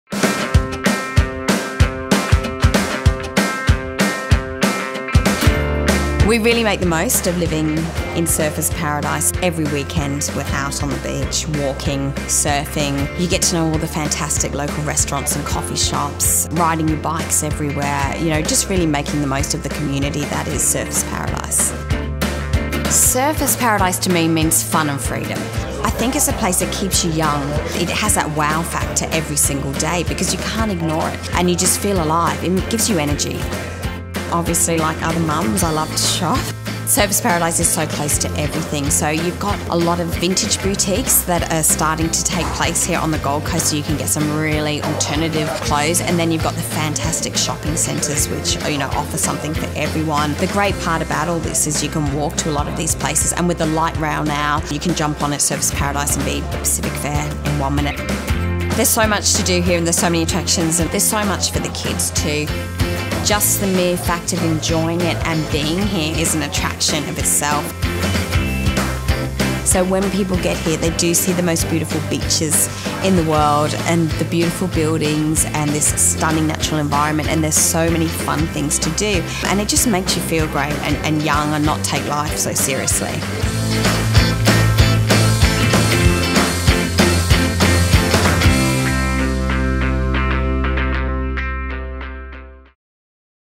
Surfers Paradise Lifestyle
“Surfers Paradise to me means fun and freedom” - Lou McGregor, a Surfers Paradise mum and local business woman. She knows how to make the most out of the precinct’s great mix of cafes, restaurants, bars, attractions, events, shopping, the beach and much more.